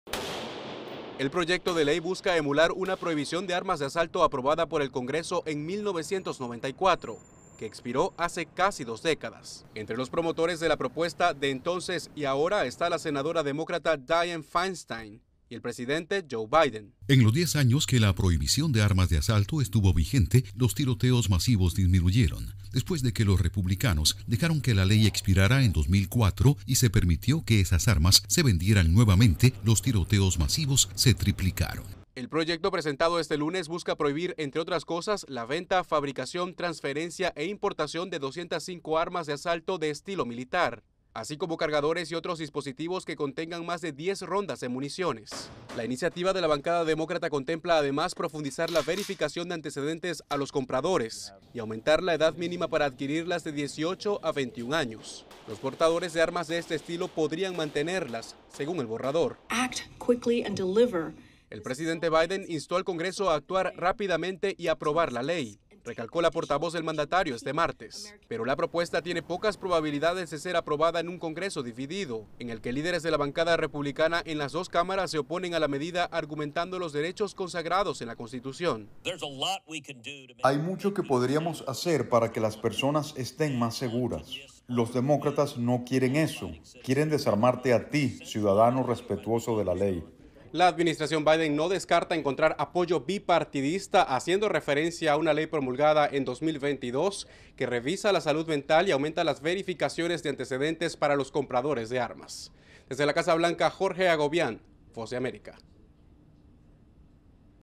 Biden renueva llamado a prohibir armas de asalto en EEUU
El presidente Joe Biden renovó el llamado al Congreso para prohibir 205 tipos de armas de asalto en Estados Unidos. El mandatario apoya una propuesta de ley presentada en el Senado por la bancada de su partido. Informa Jorge Agobian, corresponsal de la Voz de América en la Casa Blanca.